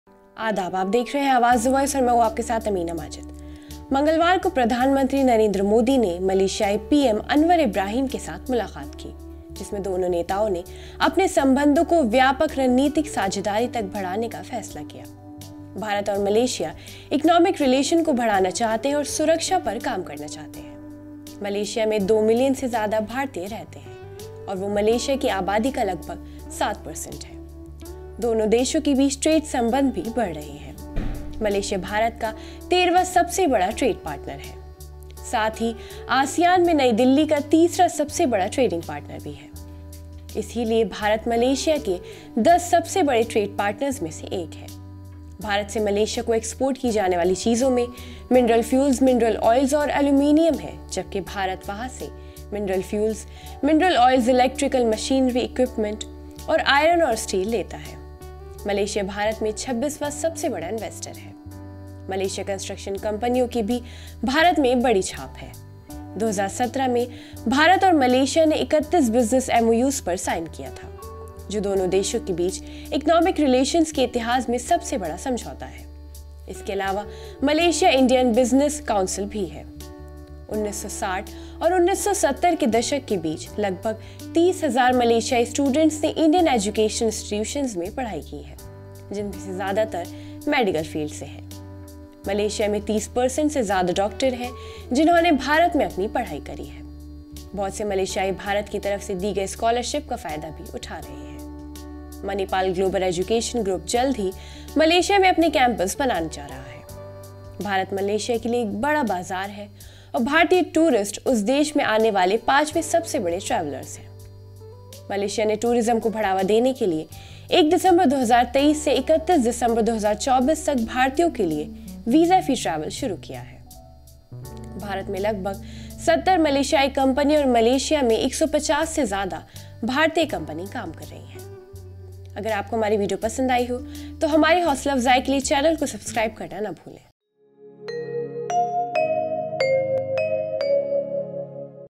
India - Malaysia Relations: व्यापक रणनीतिक साझेदारी का एक नया युग | BRICS membership
मंगलवार को प्रधानमंत्री नरेंद्र मोदी ने मलेशियाई पीएम अनवर इब्राहिम के साथ मुलाकात की। दोनों नेताओं ने अपने संबंधों को 'व्यापक रणनीतिक साझेदारी' तक बढ़ाने का फैसला किया। भारत और मलेशिया आर्थिक संबंध को बढ़ाना चाहते है  और सुरक्षा पर काम करना चाहते है । मलेशिया में 2  मिलियन से ज्यादा भारतीय रहते हैं और वे मलेशिया की आबादी का लगभग 7% हैं।